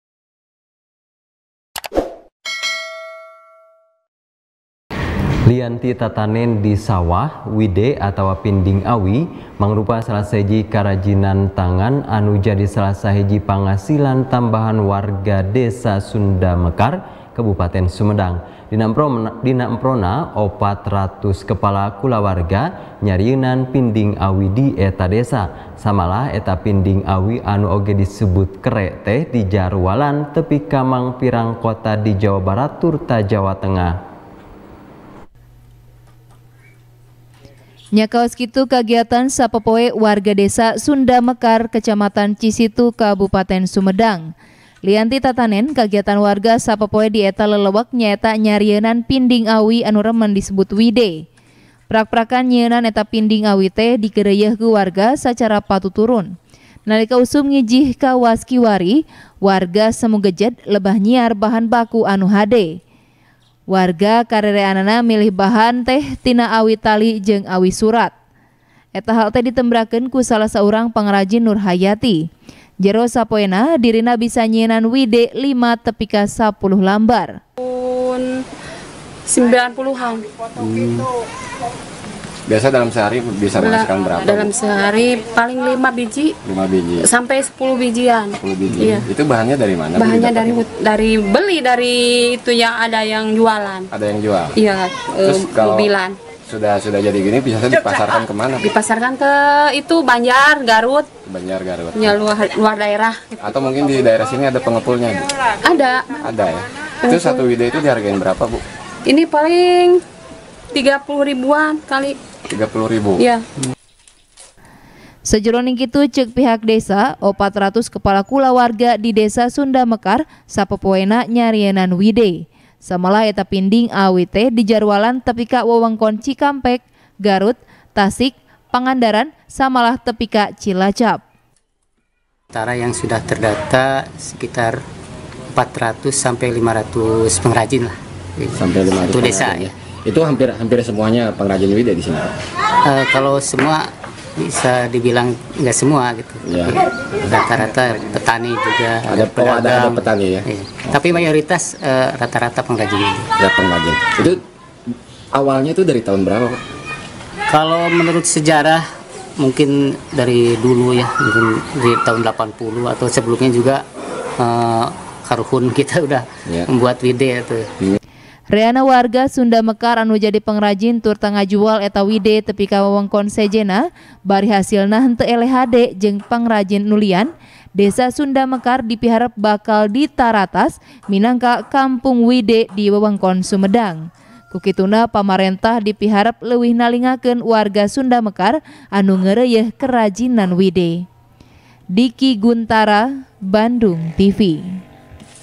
Warga Sadesa Jadi Pengrajin Pinding Awi Atawa Wide | TP | BANDUNGTV
TP | BANDUNGTV | IWAN M. RIDWAN | Lian Ti Tatanen Di Sawah, Wide Atawa Pinding Awi Mangrupa Salah Sahiji Karajinan Tangan Anu Jadi Salah Sahiji Panghasilan Tambahan Warga Desa Sundamekar, Kabupaten Sumedang. Dina Emprona, Opat Ratus Kepala Kulawarga, Nyarieunan Pinding Awi Di Eta Desa. Samalah, Eta Pding Awi Teh Dijarualan Tepi Ka Mangpirang Kota Di Jawa Barat Tur Jawa Tengah.